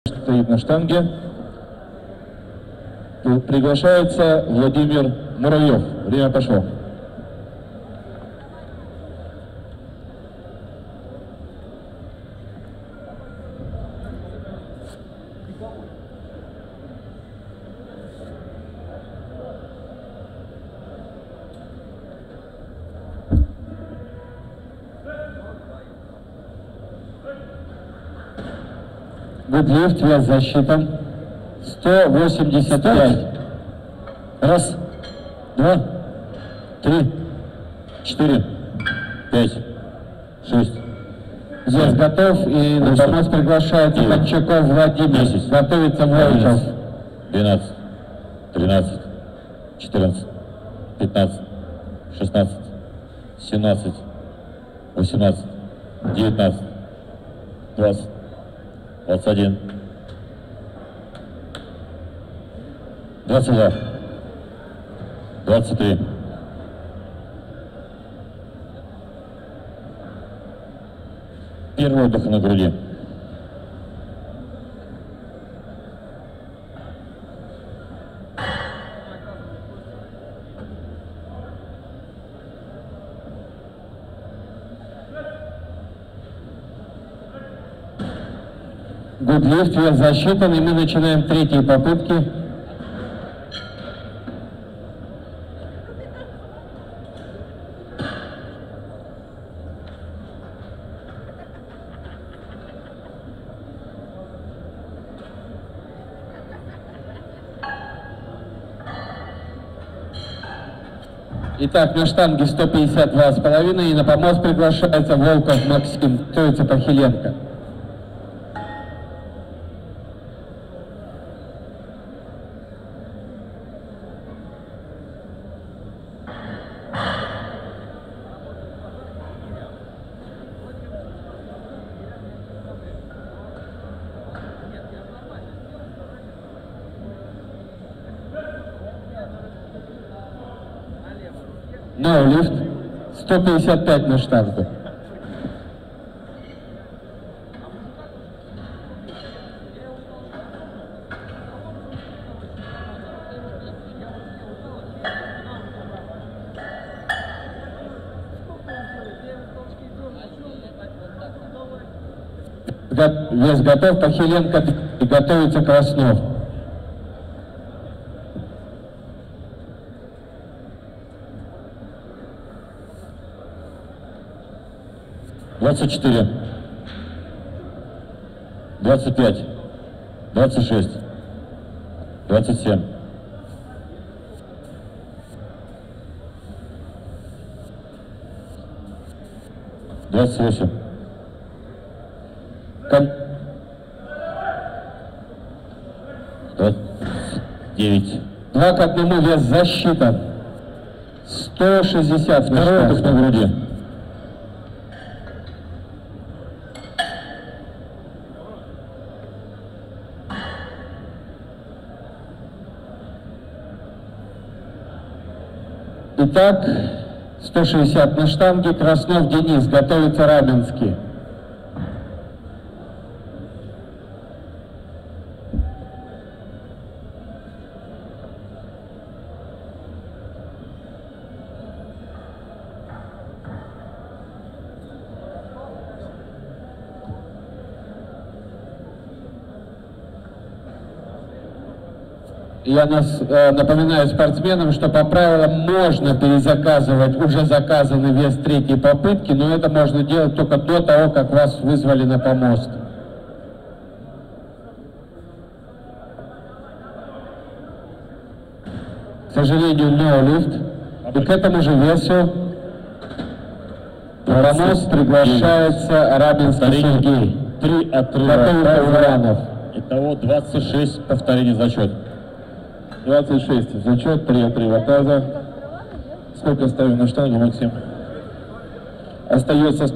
Чемпионат AWPC/WPC МЕГАПОЛИС 2013 - Народный жим WPC - Муравьёв
Чемпионат WPC МЕГАПОЛИС 2013
Муравьёв Владимир, вес 80,95 кг, 82,5х29 (26)
I м место в open и I место в masters до 82,5 кг
III место в Абсолютке masters